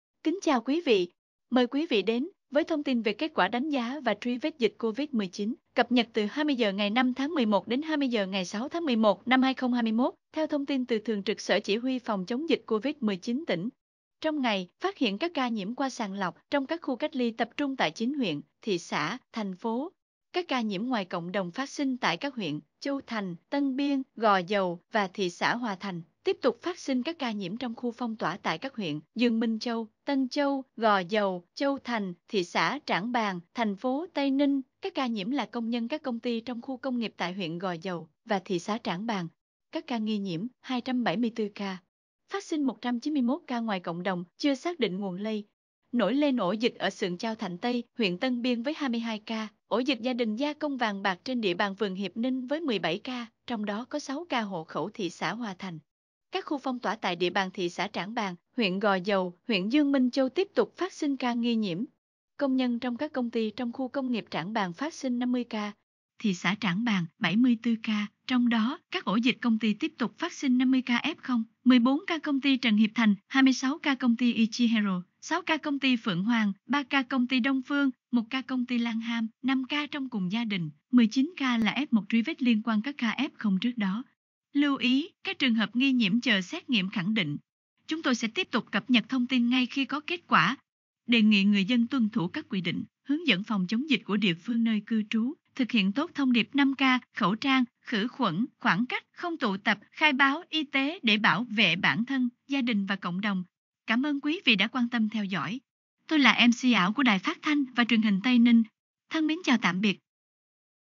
KẾT QUẢ TRUY VẾT COVID-19 TÍNH ĐẾN 20G00 NGÀY 06/11/2021 | Tin tức hôm nay | TayNinhTVAudio
Thông tin về kết quả đánh giá và truy vết dịch Covid-19, cập nhật từ 20 giờ ngày 05/11 đến 20 giờ ngày 06/11/2021.
Quý vị hãy quét mã QR góc dưới màn hình khi xem kênh truyền hình Tây Ninh TTV11 hoặc truy cập địa chỉ để nhận thông tin nhanh chóng, chính xác về tình hình dịch bệnh